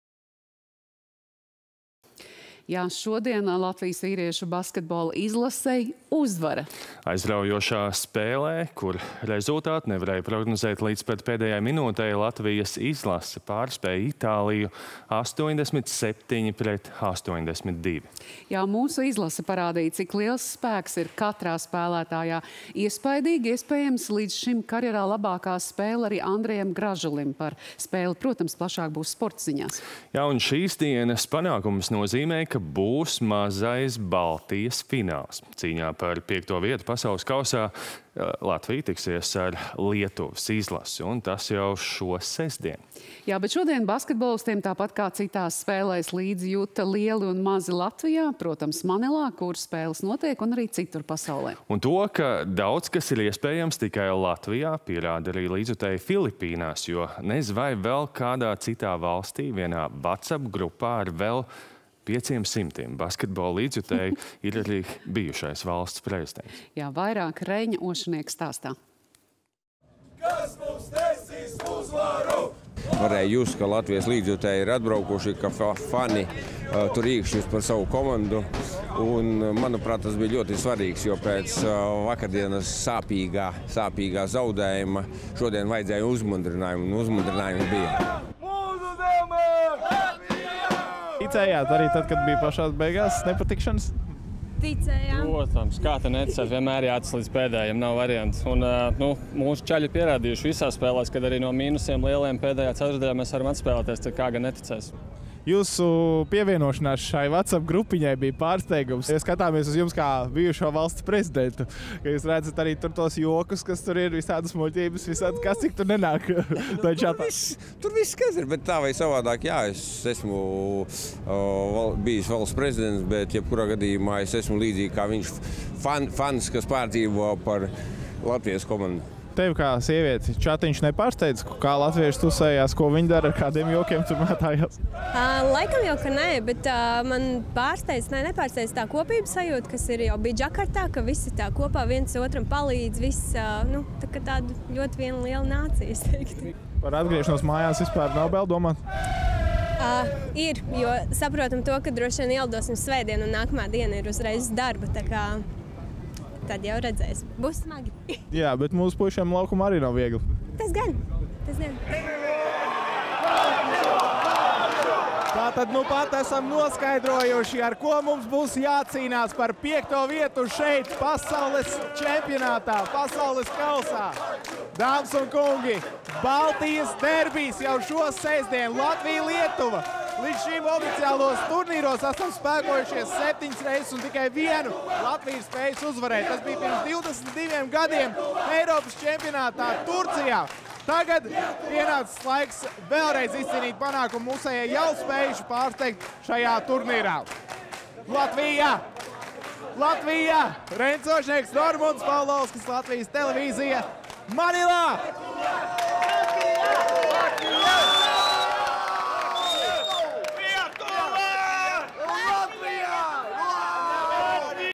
Just līdzi Latvijai – unikāla pieredze!
Šodien basketbolistiem, tāpat kā citās spēlēs, līdzi juta lieli un mazi Latvijā, Manilā, kur spēles notiek, un citur pasaulē. Un to, ka daudz kas ir iespējamas tikai Latvijā, pierāda arī līdzjutēji Filipīnās. Jo nez vai vēl kādā citā valstī vienā Whattsapp  grupā ar vēl pieciem simtiem basketbola līdzjutēju ir arī bijušais Valsts prezidents? Vairāk – Reiņa Ošenieka stāstā.